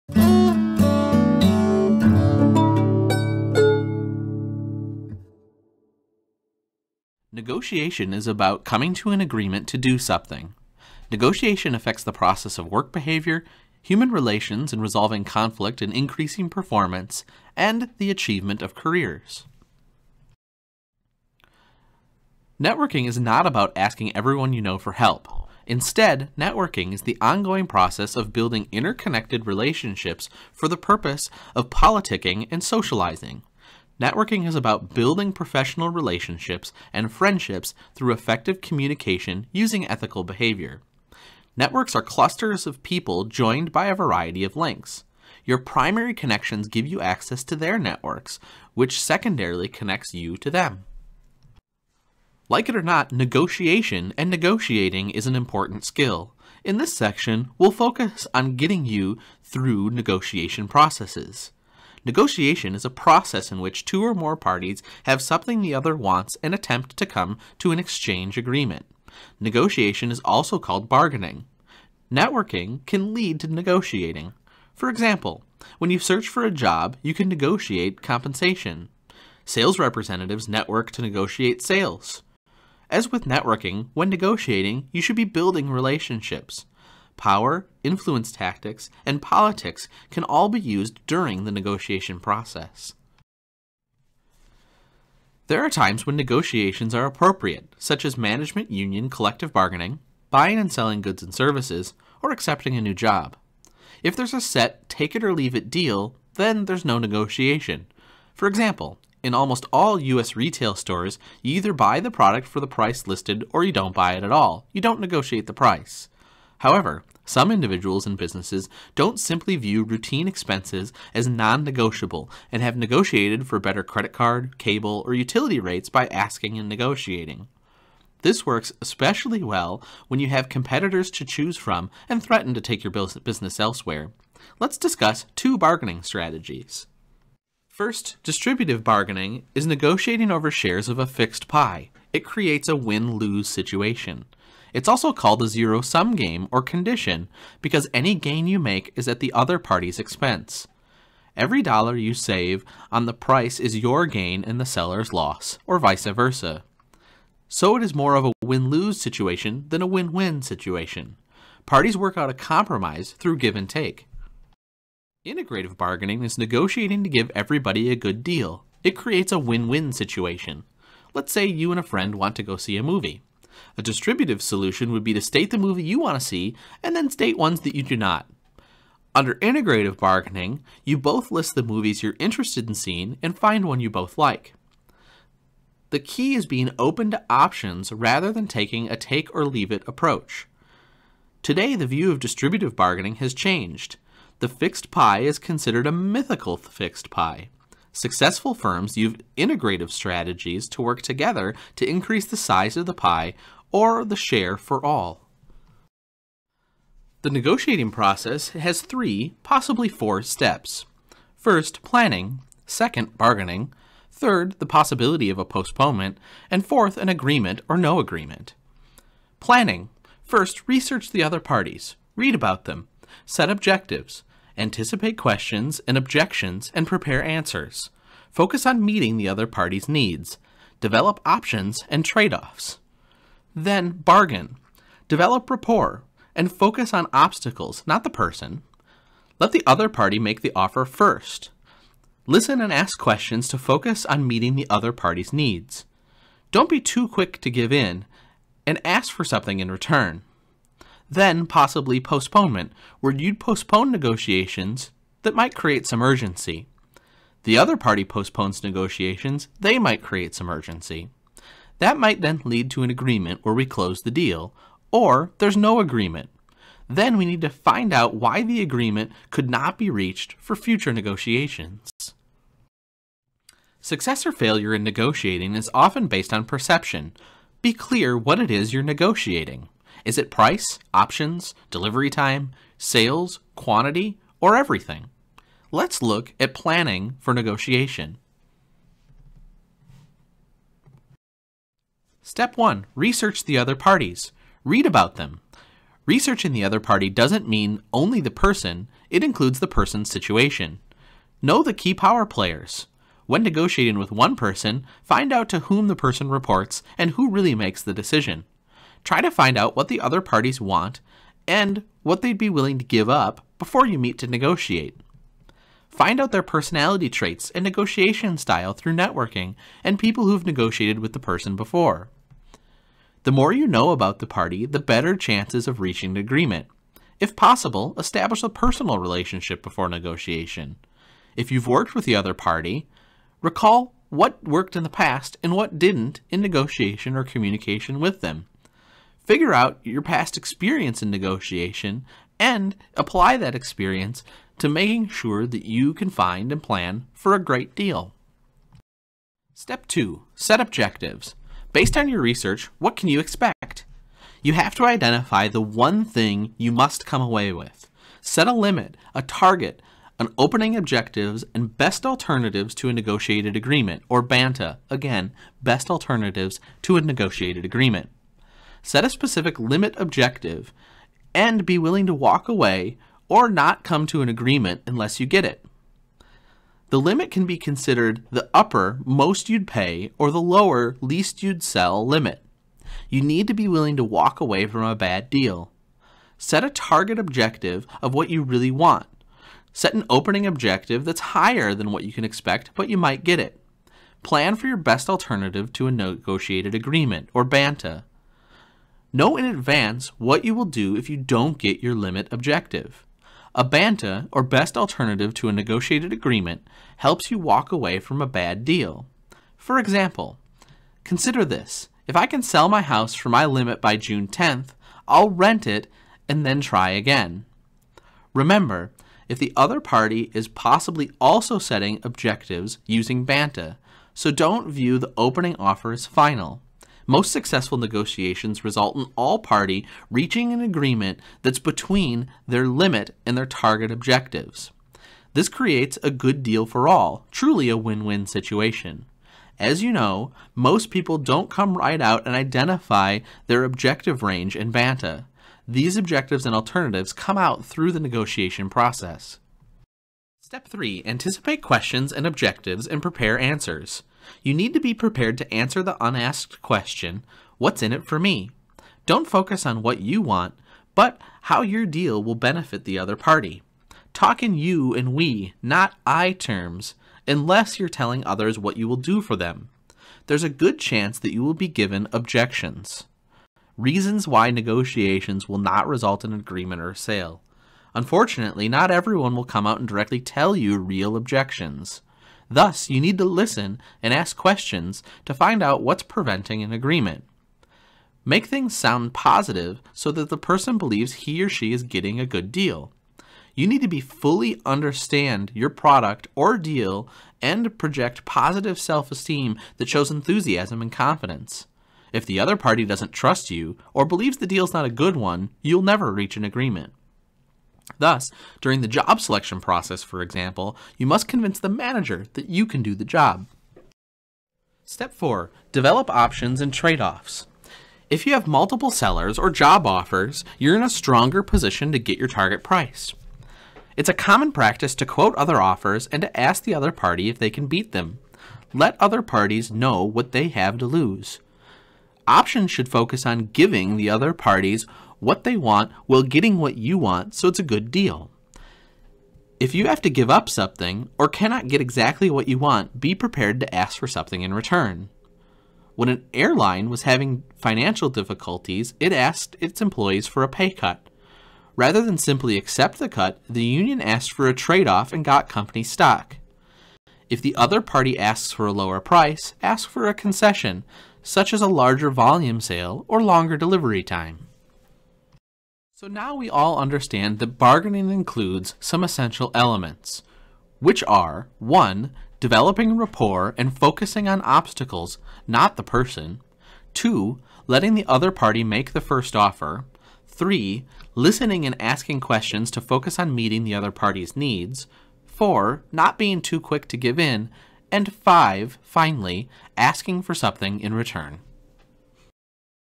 Networking and Negotiating
Negotiation is about coming to an agreement to do something. Negotiation affects the process of work behavior, human relations in resolving conflict and increasing performance, and the advancement of careers.